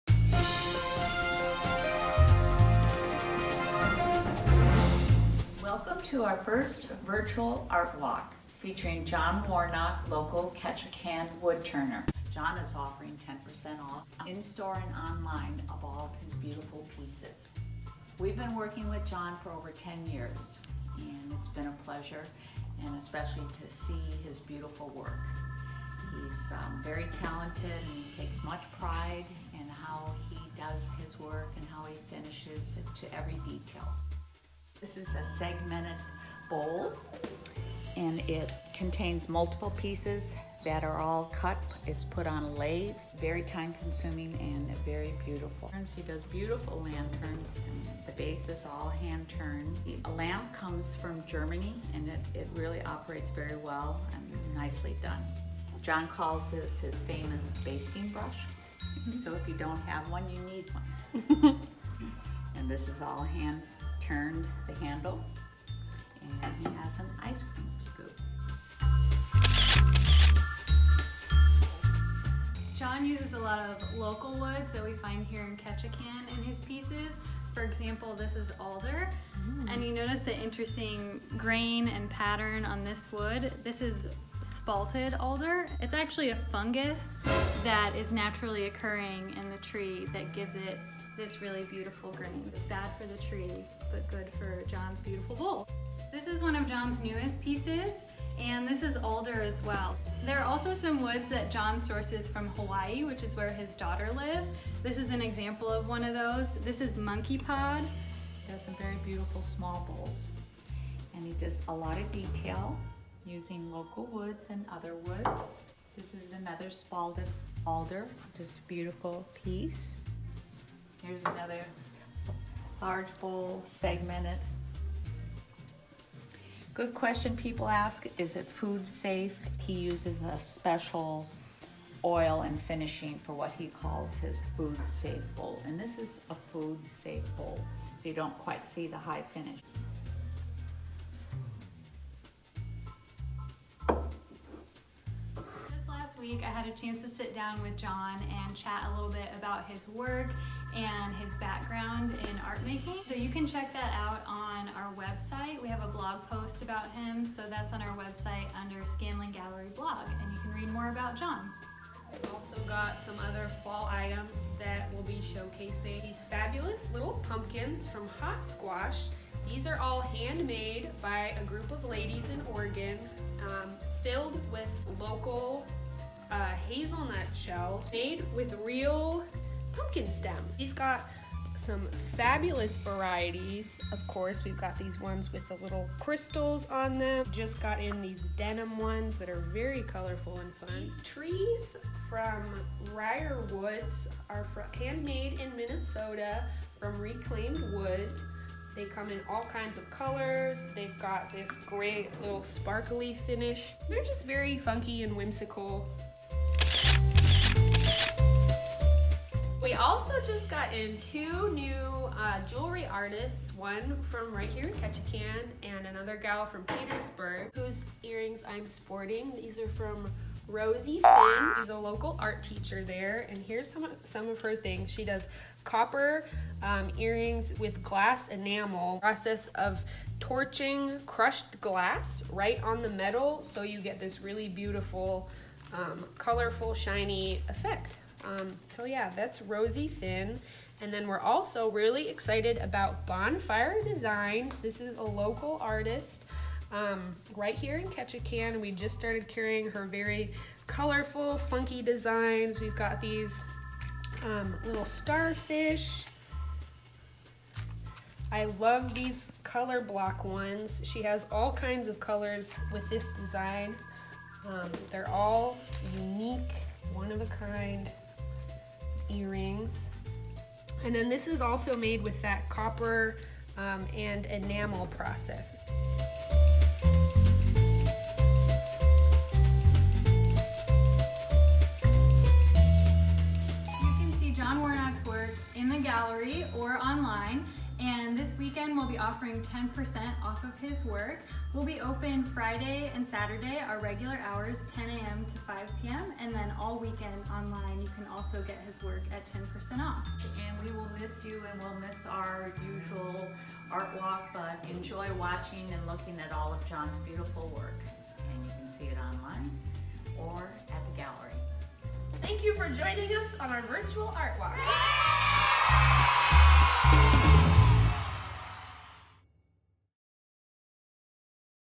Artober 2020 Virtual Art Walk at Scanlon Gallery
Join us at the gallery to see the woodturning of Ketchikan artist John Warnock and other new items for fall!